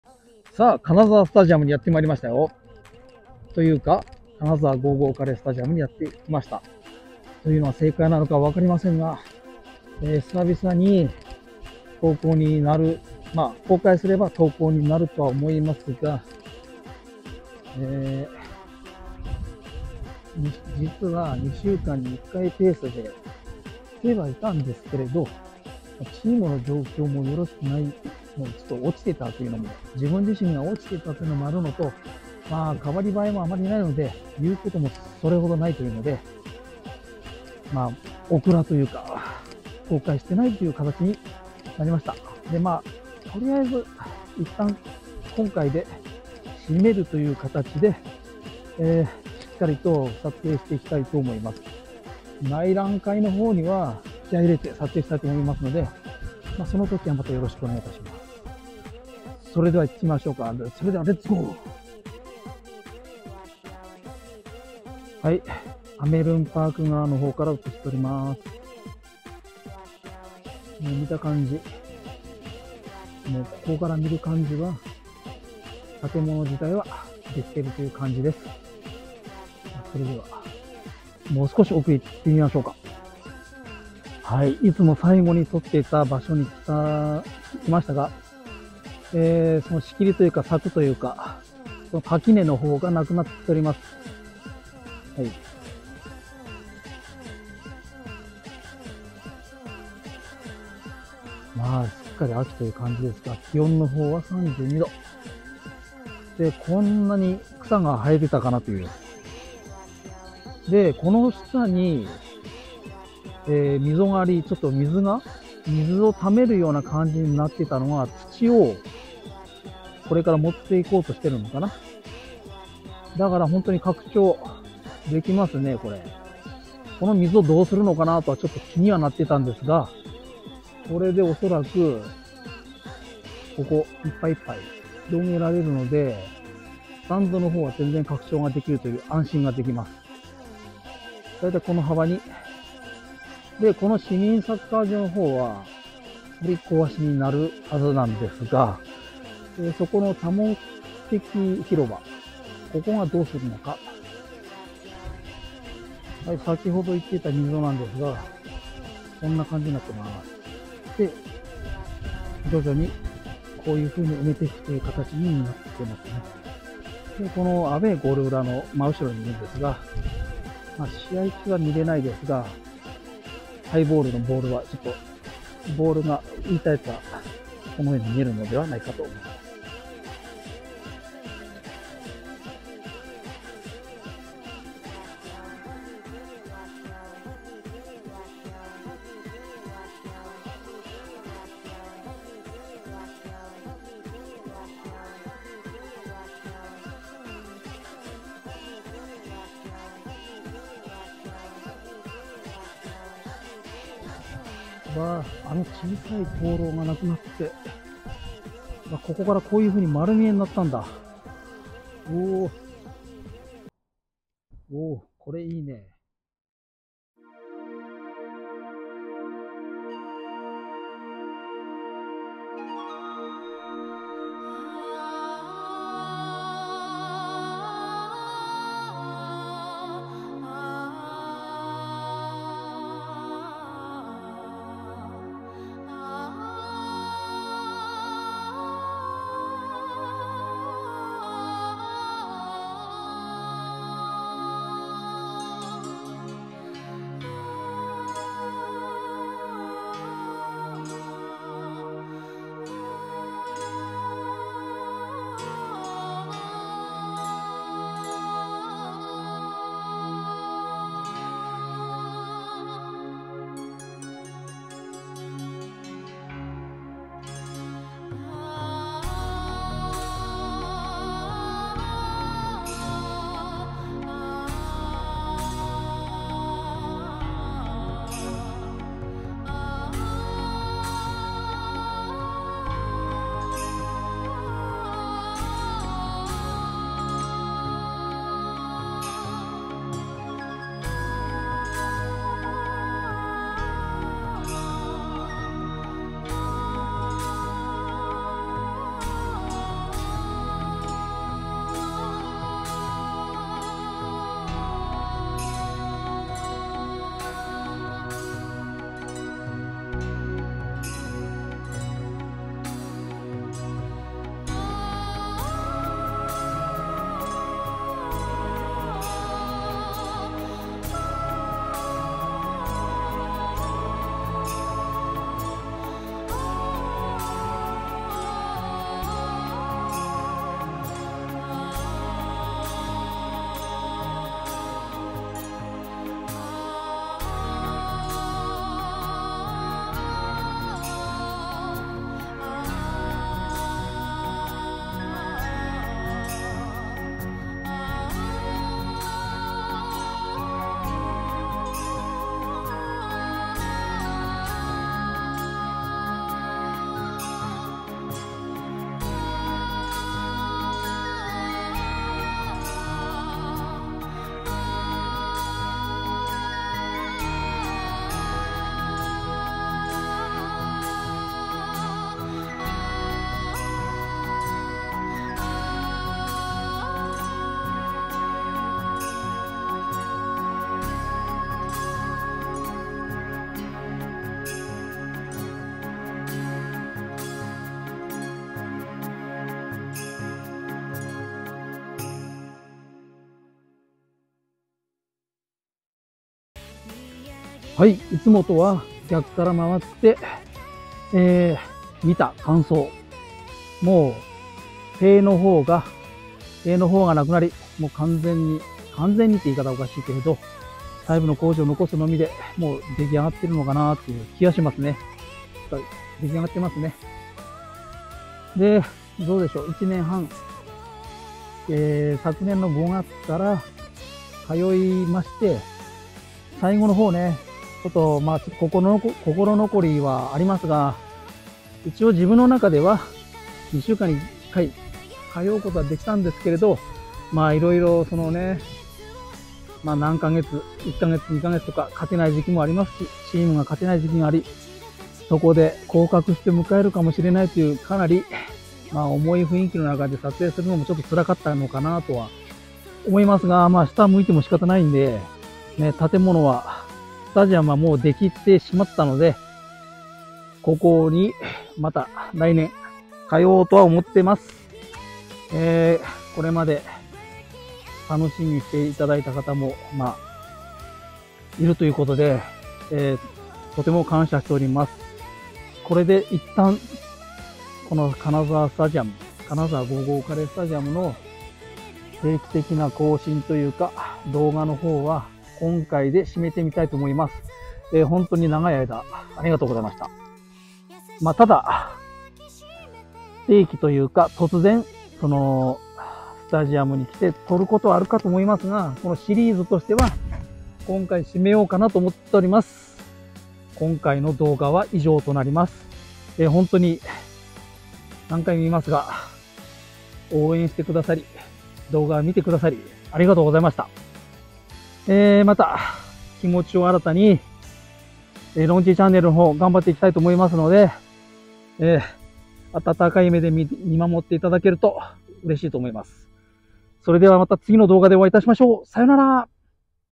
俺たちの新スタfinal #36
金沢スタジアムを1年半撮り続けて来ましたが今回で終わると思います。
最後の１ヶ月は公開出来なかったのが残念ですが今後不定期で撮る事もあると思います。
ワイヤレスマイクの不調で音声が悪くてすいません🙏